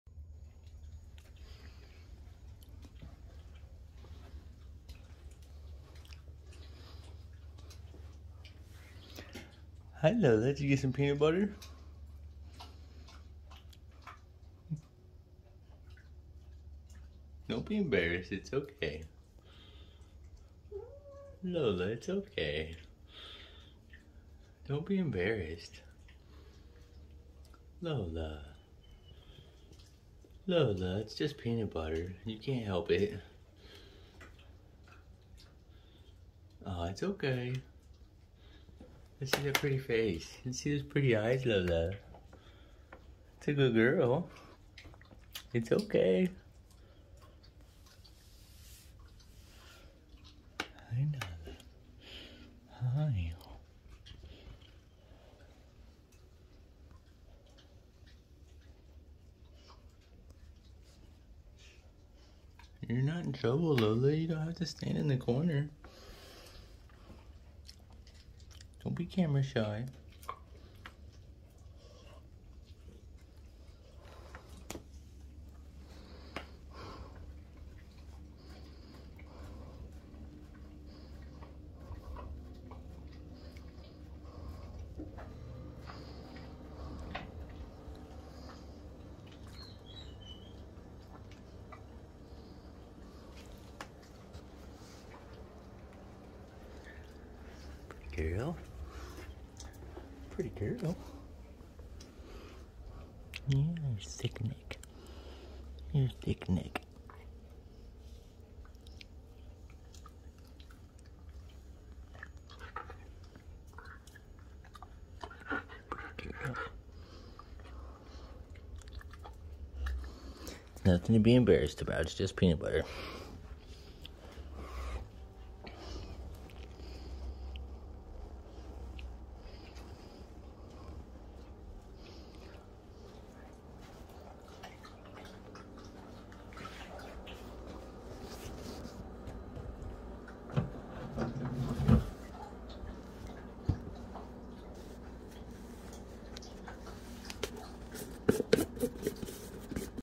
Siberian Husky is camera shy and embarrassed about eating peanut butter
Siberian Husky Lola loves peanut butter but is embarrassed when eating it